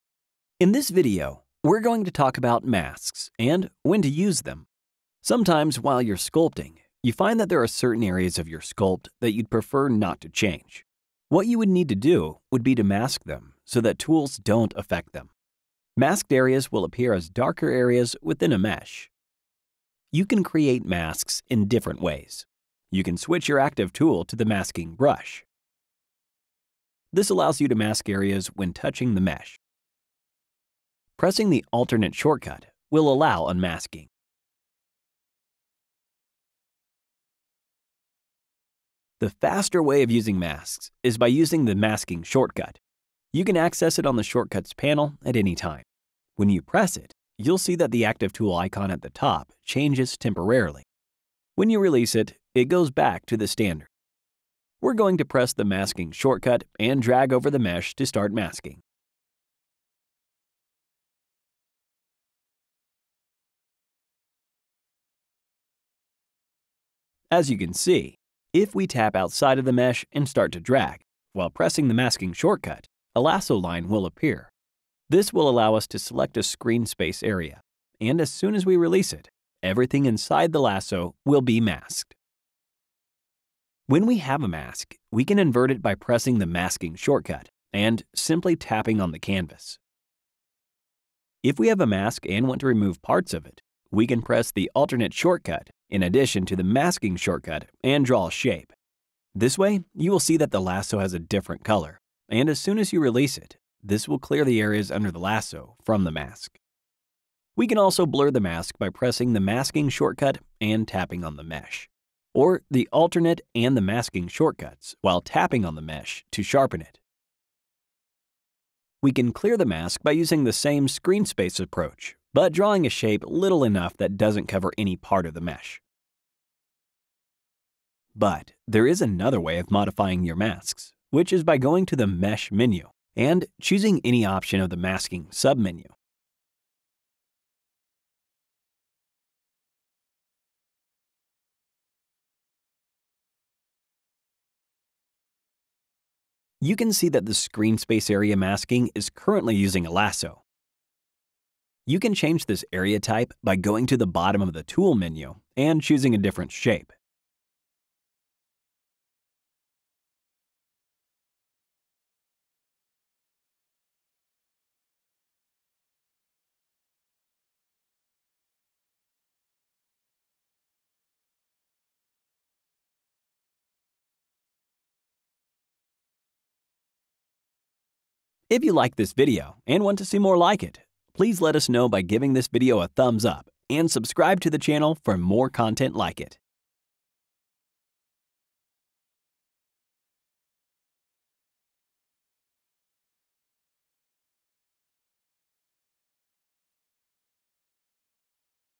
forger iPad - Sculpting - Masks
0:00​ Intro
0:24​ Masking Brush
0:42​ Masking Shortcut
1:07​ Screen-space masking
1:23​ Inverting masks
1:31​ Removing parts of the mask
1:46​ Blurring and sharpening masks
1:58​ Clearing masks
2:15​ Masking Submenu
2:30​ Changing the screen-space masking
2:58​ Outro